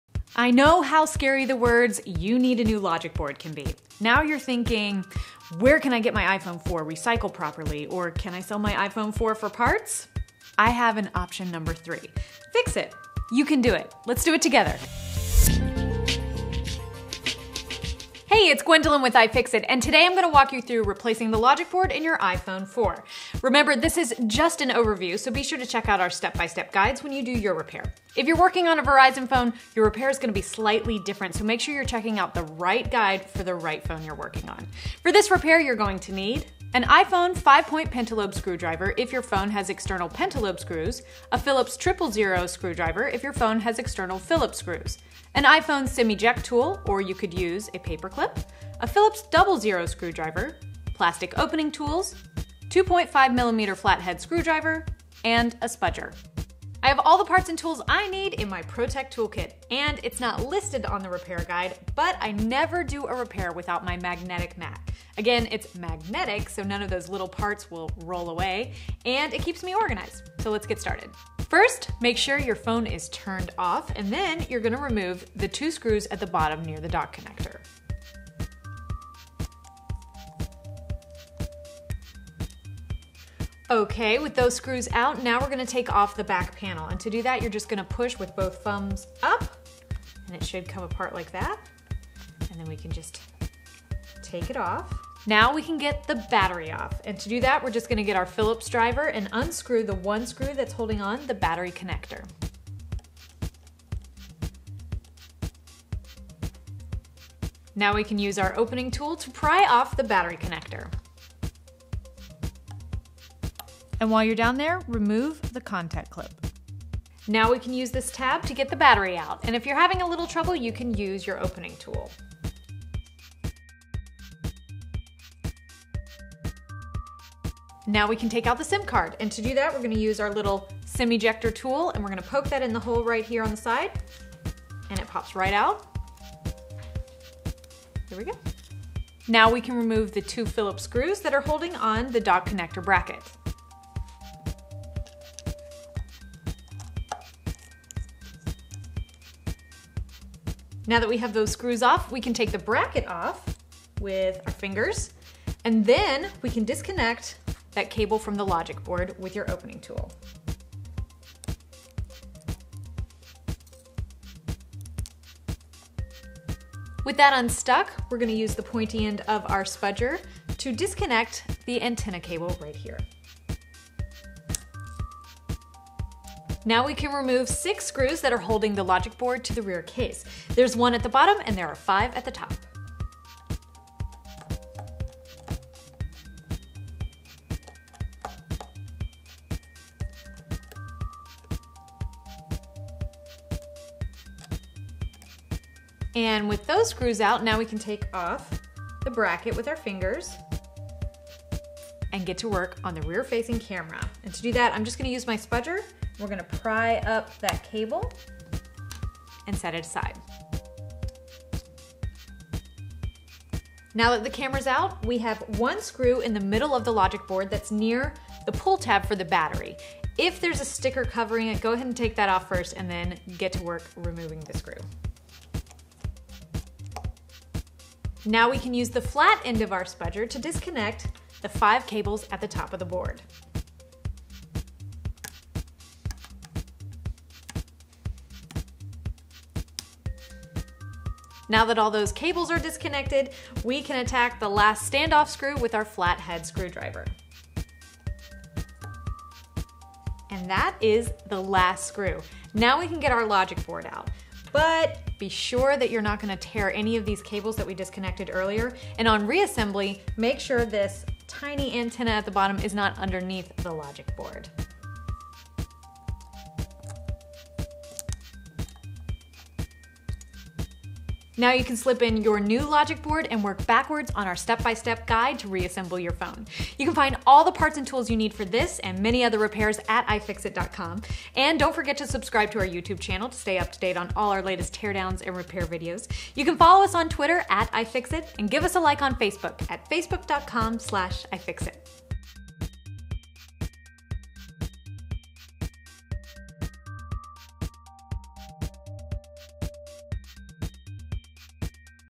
How To: Replace the iPhone 4 Logic Board
We know how scary the words "you need a new logic board" can be the first time you hear it. Now you're thinking "Who can I sell my iPhone 4 to?" or "Where can I get my iPhone 4 recycled properly?" . We are here to propose option number three. Fix it!

Get your replacement Logic Board
GSM